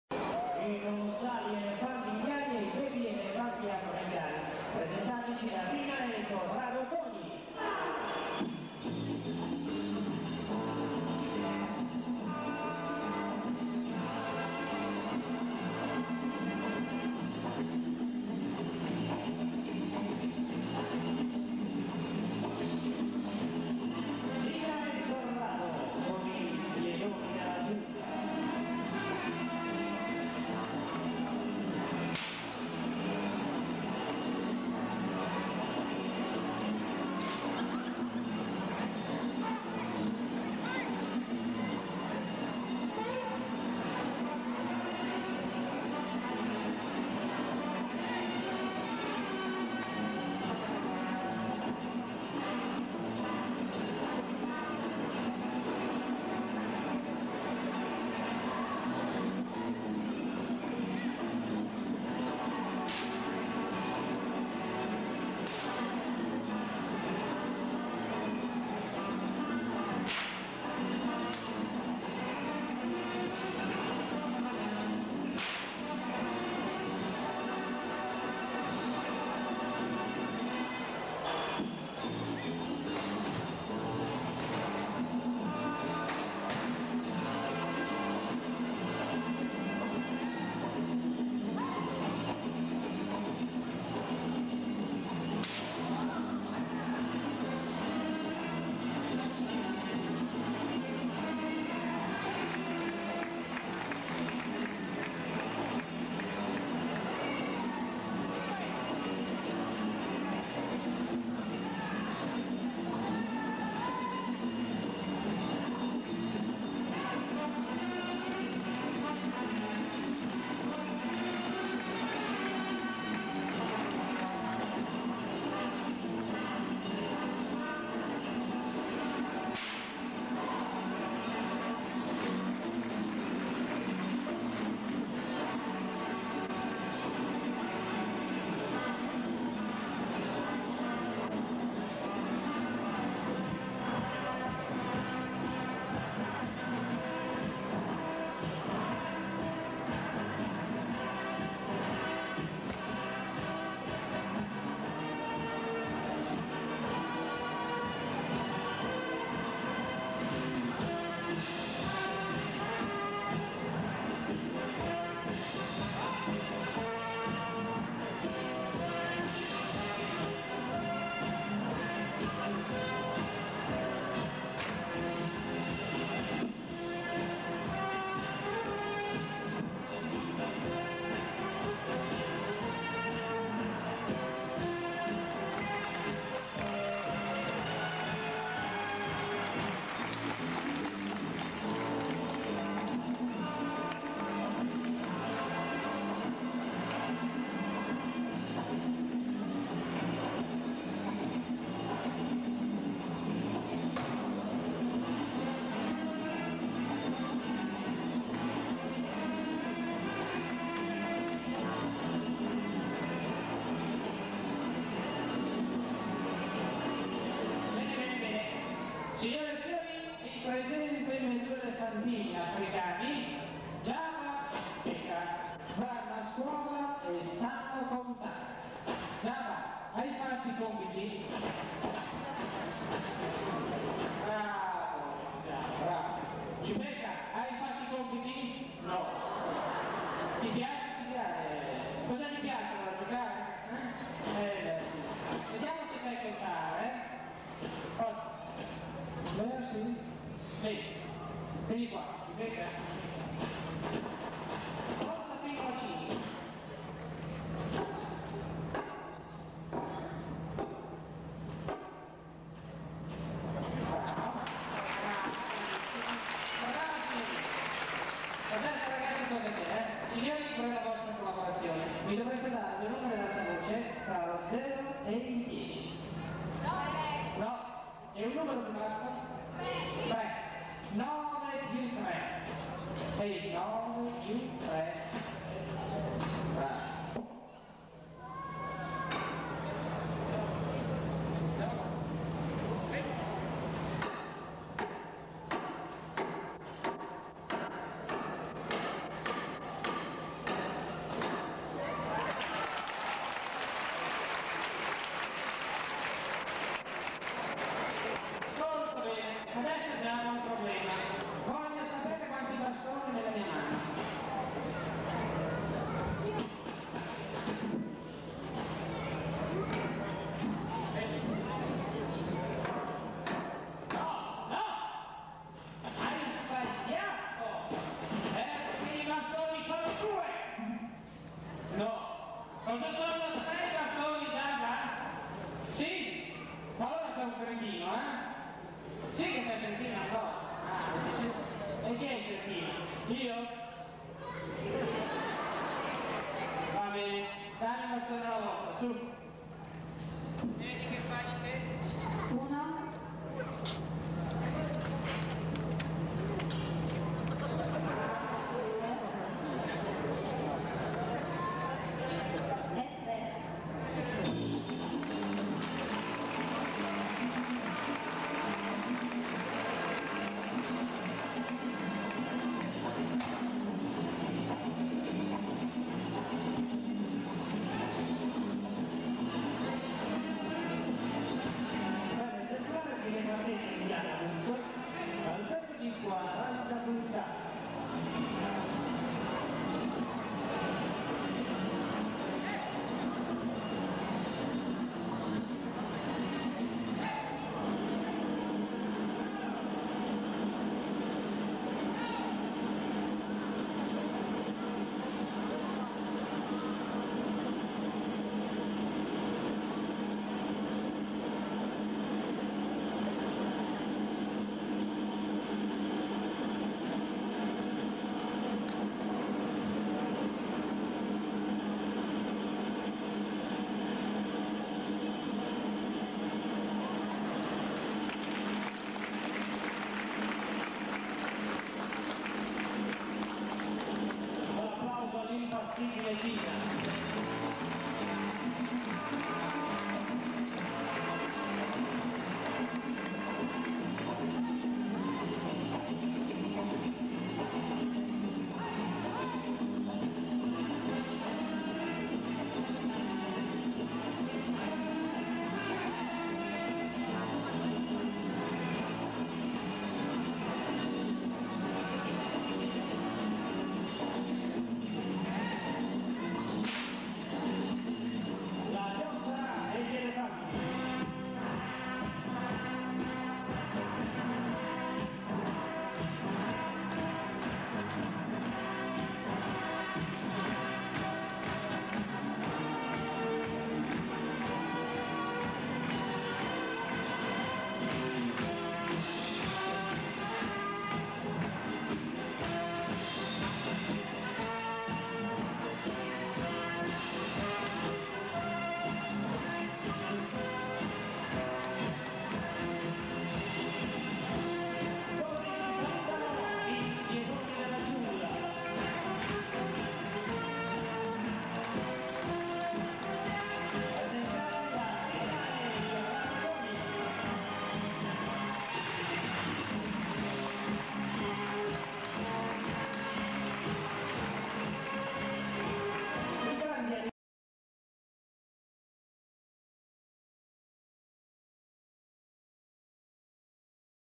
Gli elefanti di Corrado Togni (Circo Darix Togni, 1986)
Nel 1986 il Circo Darix Togni si presenta con un ottimo parco animali e uno stile di spettacolo molto personale. Siamo alla vigilia del salto che nel giro di circa 4 anni li porterà alla nascita del Florilegio. Nella primavera 1986 il circo di Livio, Davio e Corrado Togni agisce in Lombardia, in provincia di Varese sotto ad uno chapiteau a righe. Anche il parco animali negli ultimi periodi si è arricchito notevolmente: l’esotico di Davio porta in pista ippopotamo, rinoceronte (cavalcato!), giraffa, bisonti ed esempleri di altre razze. E Corrado, con Tina Palacios, porta in pista un gruppo di cinque elefanti: 3 indiani e 2 africani; questi ultimi due “calcolatori”, ossia un gioco da sempre presente negli spettacoli di Darix Togni, sin dai tempi di Ugo Miletti, in cui un elefante risponde ai calcoli matematici proposti dall’addestratore. Vi proponiamo il numero completo tratto dal vasto archivio storico di Mauro Cantoro.